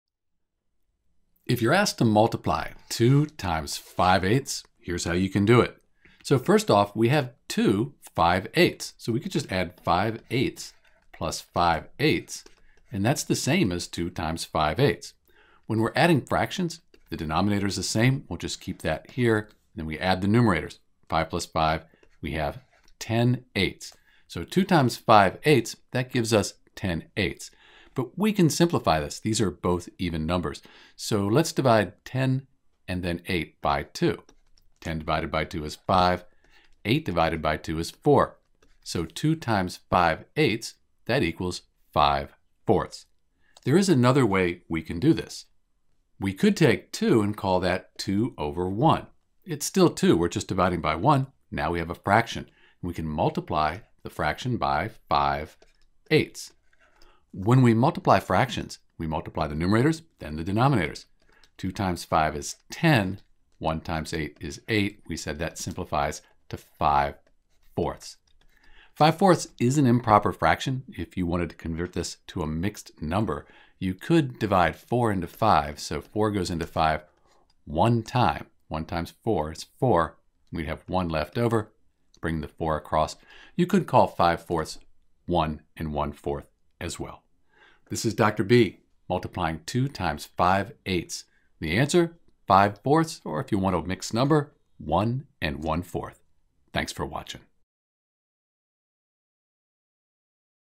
2 times 5/8 (Two times Five-Eights)
When we multiple a number by a fraction we first multiply the numerators and then multiple the denominators.  In this sense multiplying a fraction like 2  x 5/8 is fairly easy.

The trick is to convert 2 to 2/1 so we have a fraction.  Since we are dividing by "1" we haven't change the value, just the way it is written. We then have:
2/1  x  5/8

We multiply the numerators:  2 x 5 = 10
and then the denominators:   1 x 8 =  8

We end up with 10/8 which we can simplify to 5/4.